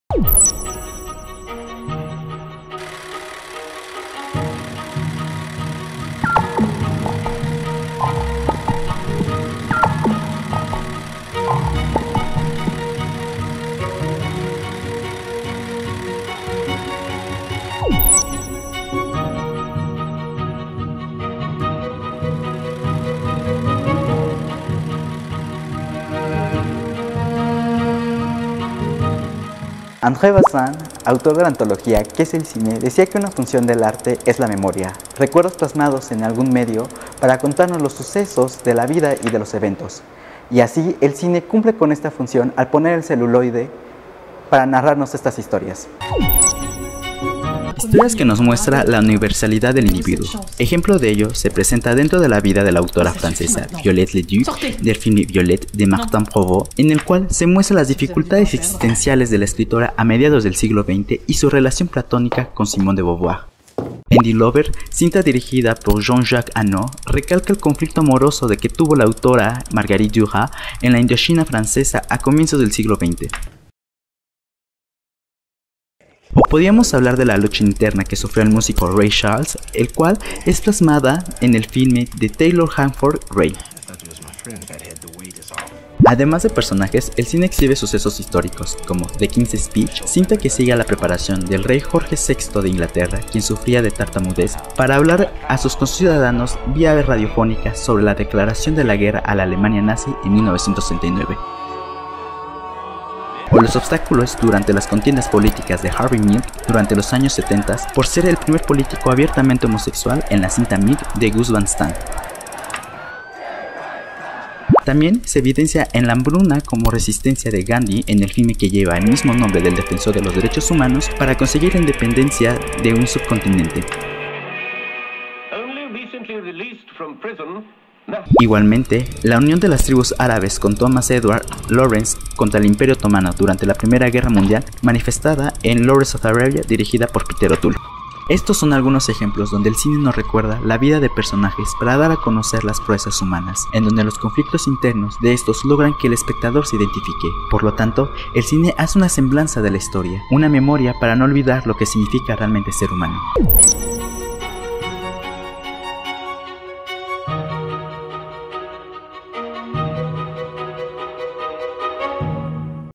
La vida de personajes históricos - Fotogramas
Explora con nosotros el emocionante mundo del séptimo arte en "Fotogramas". Esta semana analizamos la vida de algunos personajes históricos y sus hazañas, a través de cintas como "The King’s Speech", "Lawrence of Arabia", "Milk", "Violette" y más.